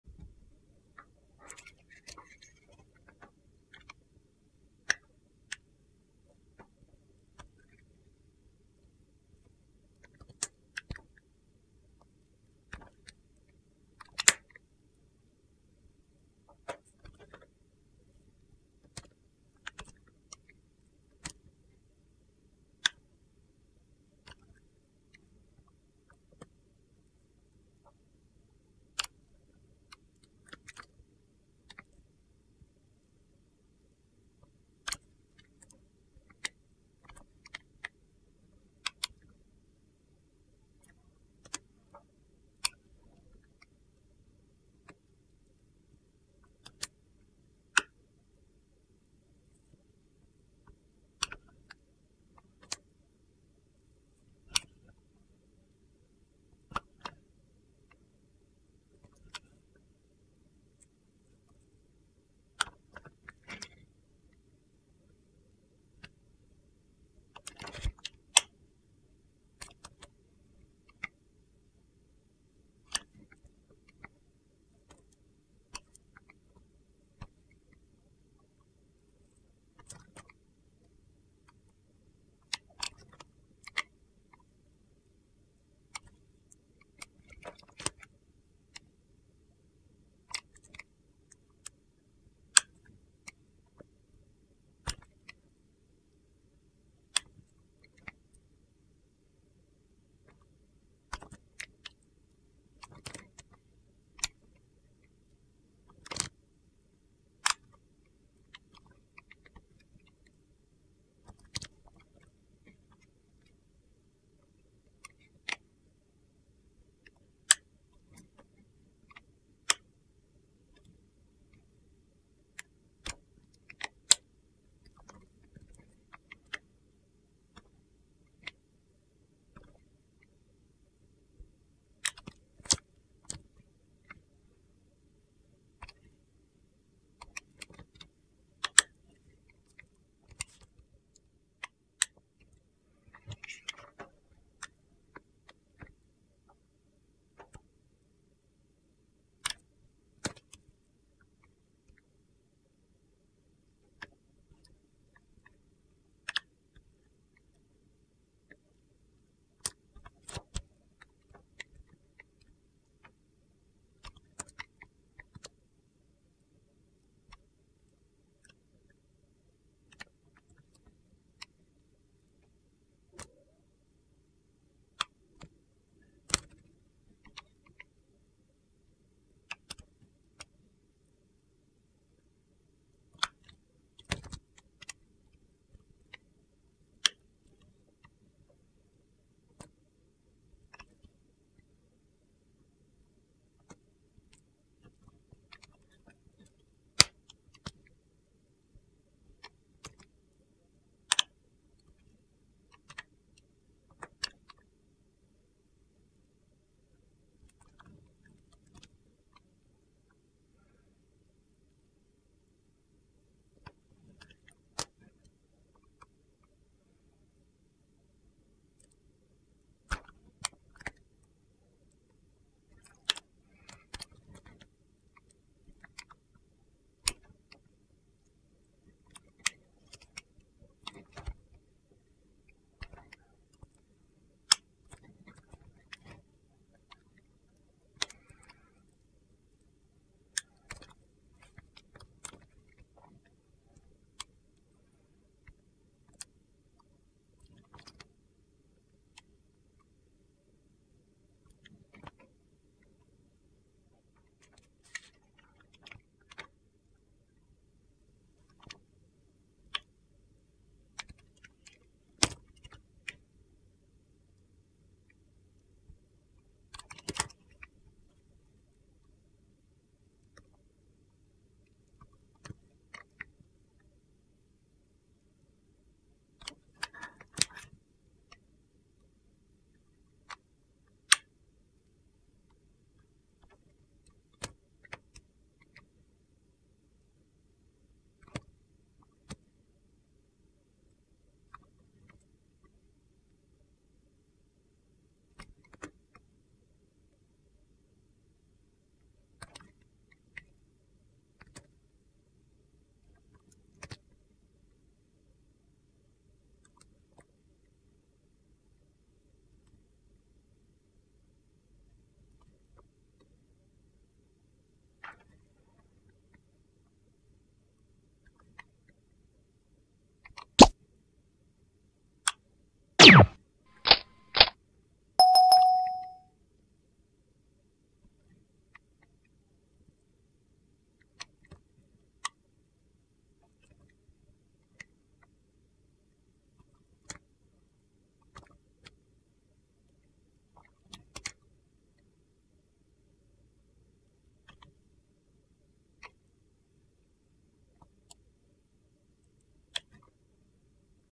ASMR Soap Carving Relaxing Sounds !Satisfying Crunchy Gritty Tapping Sound No Talking ASMR Unlimited
ASMR Soap Carving ASMR UNLIMITED Relaxing Sounds ! Satisfying Crunchy Gritty Tapping Sounds (No Talking)
These soaps are so cute and mesmerizing!  It's like cutting and slicing little cakes that I don't want to end especially on the soap cubes and soap curls. I feel relaxed after making these videos.

Hope you enjoy them as much as I do!